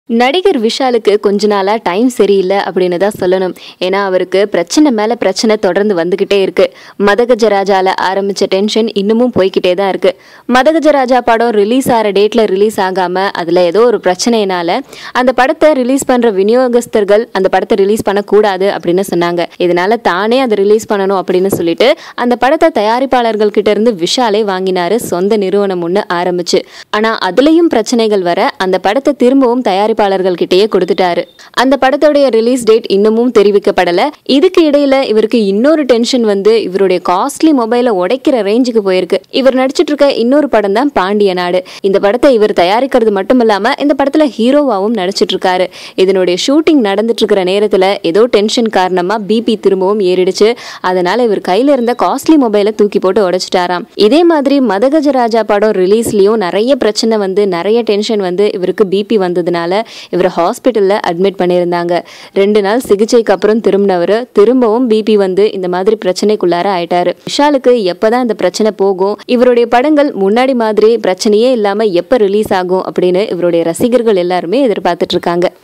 Vishal out of control, breaks his mobile phone | Pandinadu | Tamil Cinama News
Looks like taking on multiple roles on the set of his film has taken a toll on Vishal. The actor, who is also producing his next, Pandiya Nadu, came under a lot of pressure recently and, in a moment of frustration, smashed his costly mobile phone to the floor and damaged it!

A source says, "Vishal has been concentrating on Suseenthiran's film now, and has been giving his everything for it. While shooting for a scene a couple of days ago, the actor lost his cool. In a fit of despair, he flung his costly mobile phone and broke it."